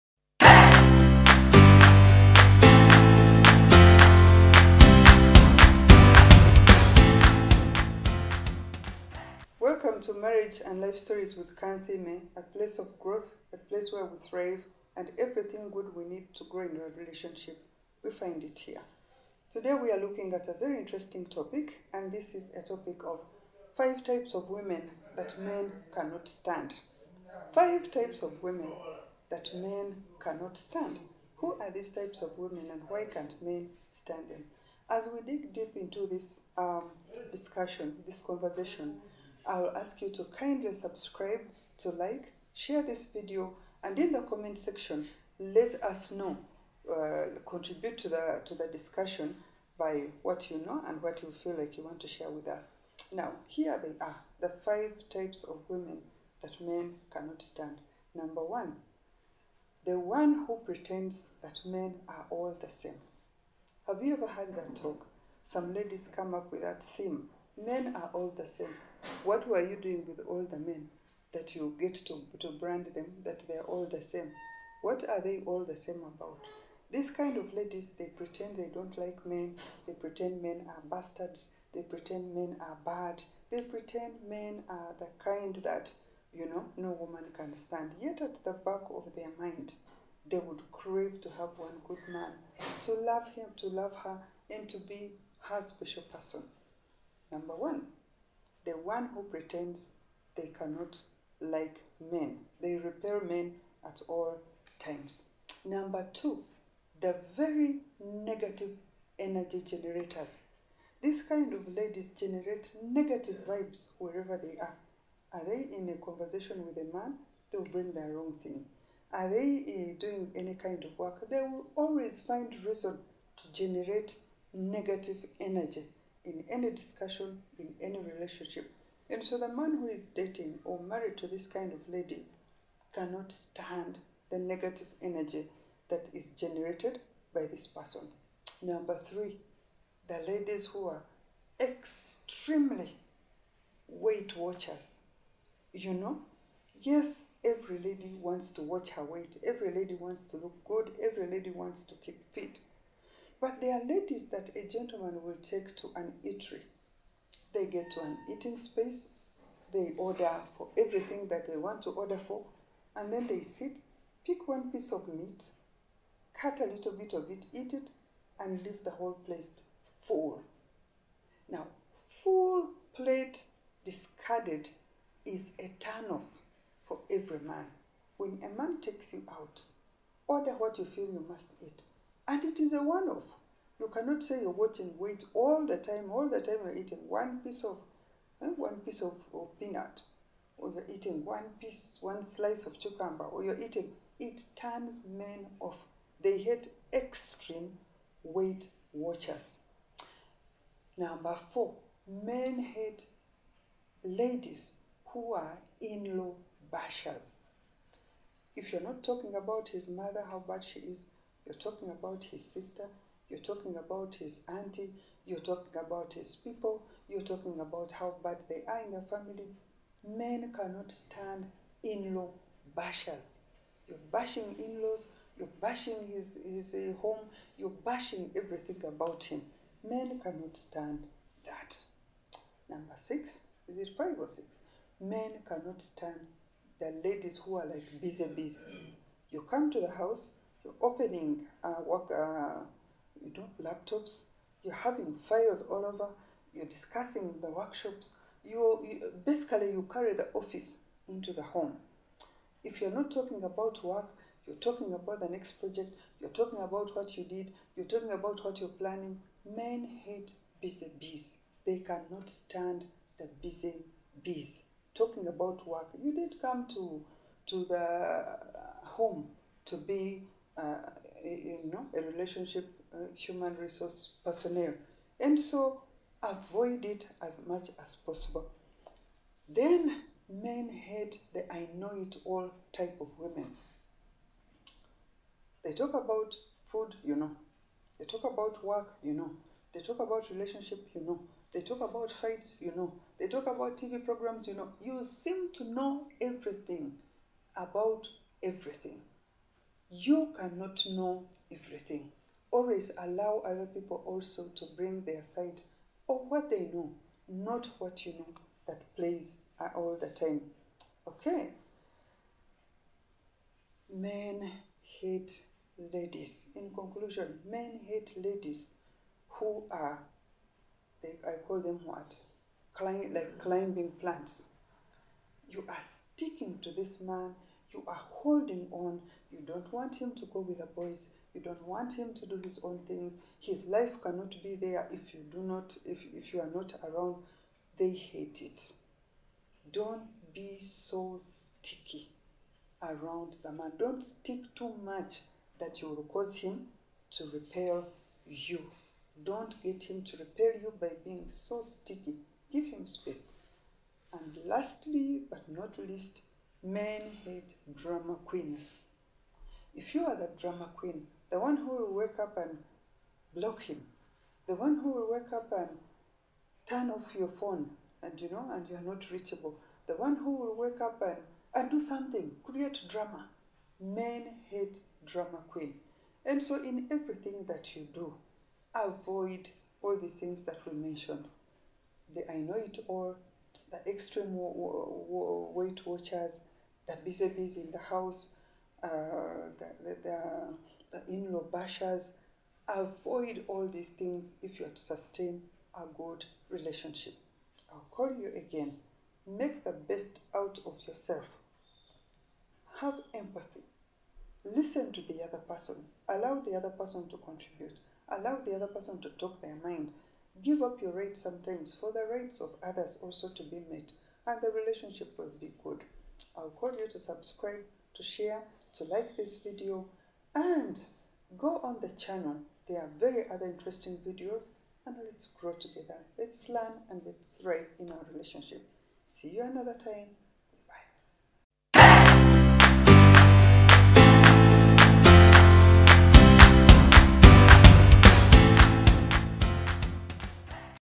Why Men Walk Away From Relationships || 5 Types Of Women That Men Can't Stand - Kansiime
Some men walk away from relationships after a short while, who are the type of women that men can not stand? ever heard of drama queens & in law bashers? watch the video as i share my opinion